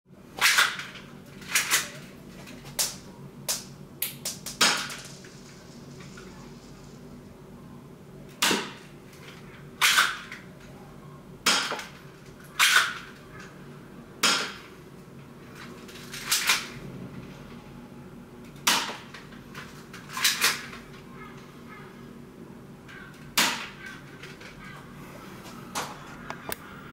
An airsoft dual wielding target shooting practise
I used the Walther P22 and the Walther P99.

-This is a minus 0,08 joule airsoft gun,because you need a license in my country for airsoft replica's which shoot harder then 0,08 joule.